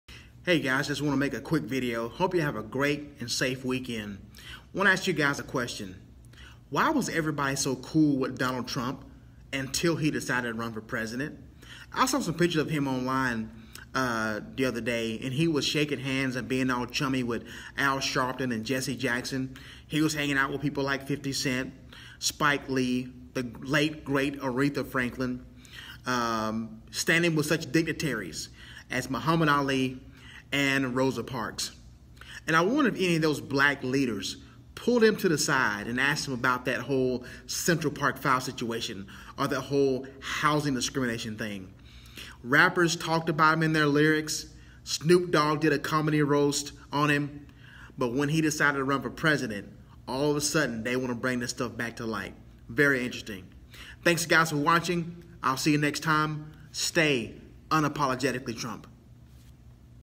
Donald Trump A Racist.... REALLY?
It’s funny how people were cool with our president before he became president.